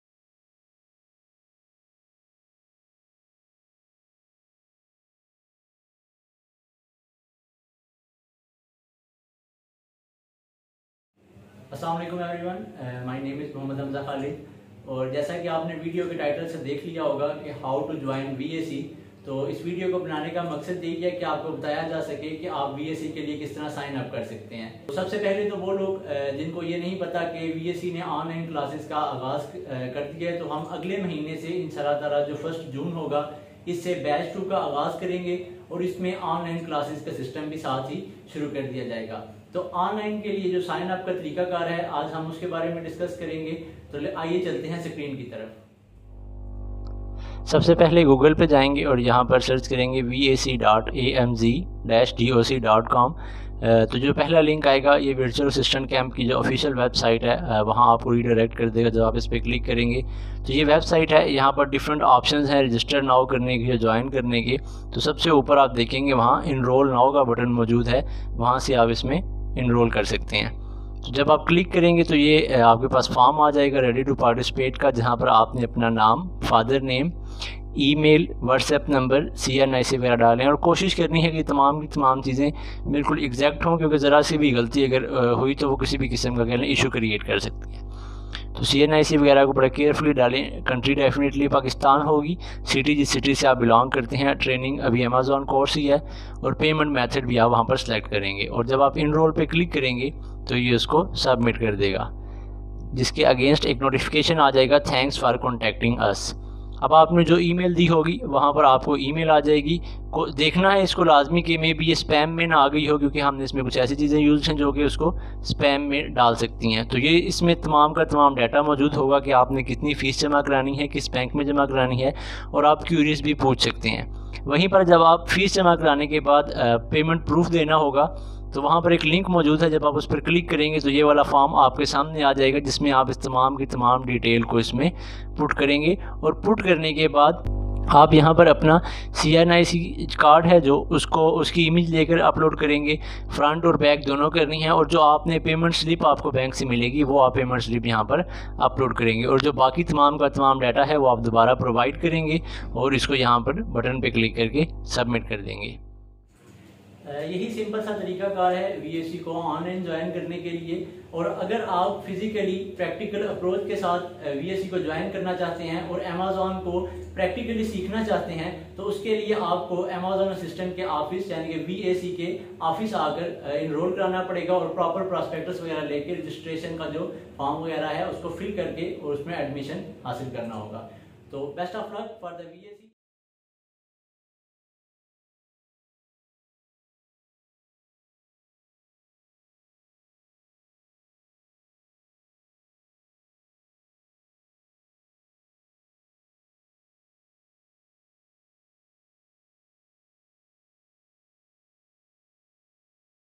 How to Sign-up VAC | Virtual Assistant Camp
Virtual Assistant Camp (VAC) is a Project by Amazon Assistant Pvt Ltd. In which we are working on providing knowledge & training about Amazon & related E commerce platforms.